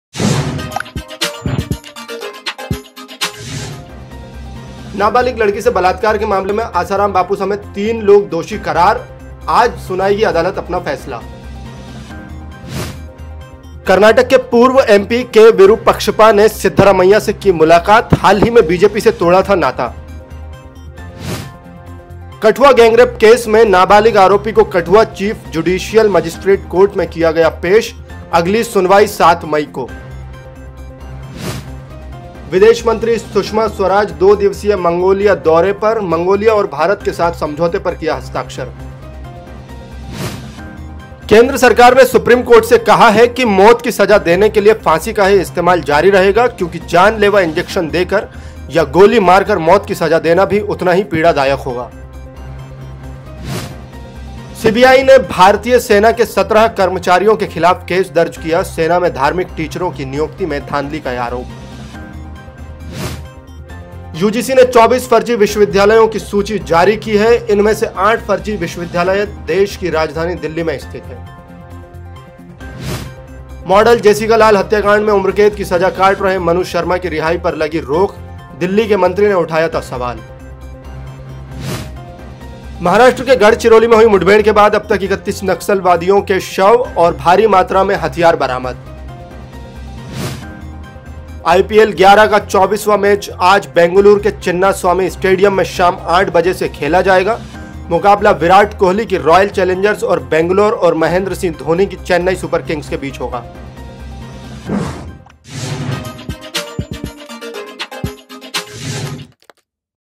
Lokmat News Afternoon bulletin: Today's complete news roundup
नाबालिग लड़की से बलात्कार के मामले में आसाराम बापू समेत तीन लोगों को जोधपुर की स्थानीय अदालत दोषी करार दिया है। अदालत आज ही दोषियों को सजा सुनाएगी। अदालत ने दो अभियुक्तों को आरोपों से बरी कर दिया है।
कर्नाटक के पूर्व एमपी के. विरुपक्षप्पा सिद्धरमैया से की मुलाकात। हाल ही में बीजेपी से तोड़ा था नाता। 12 मई को होने वाले हैं कर्नाटक विधान सभा चुनाव के लिए मतदान